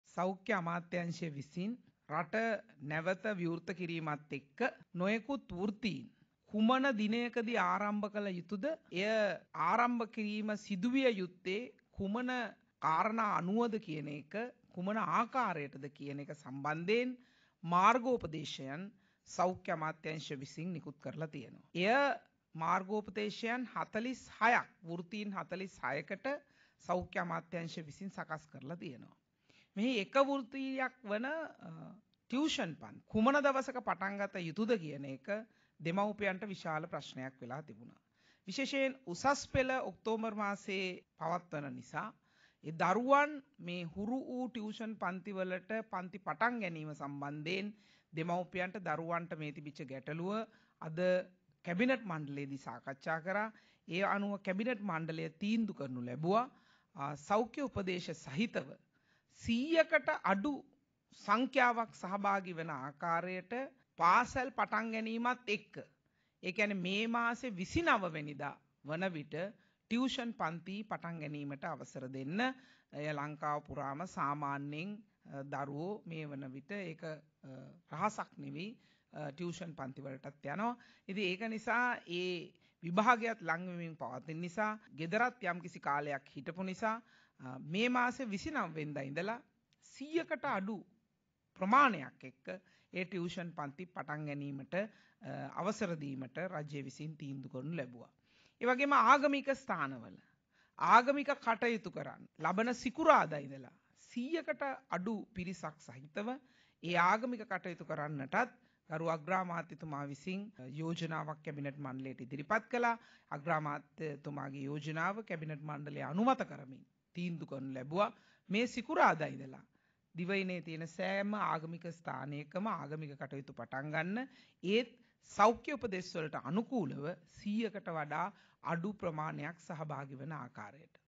ටියුෂන්, ආගමික කටයුතු සඳහා අවසර
පාසල් විවෘත කිරීමත් සමග ටියුෂන් පන්ති ආරම්භ කළ හැකියි. 12 වැනි දා සිට ආගමික කටයුතු සඳහා ඉඩ ලැබෙනවා. හැබැයි 100 දෙනෙකුට යටත් ව..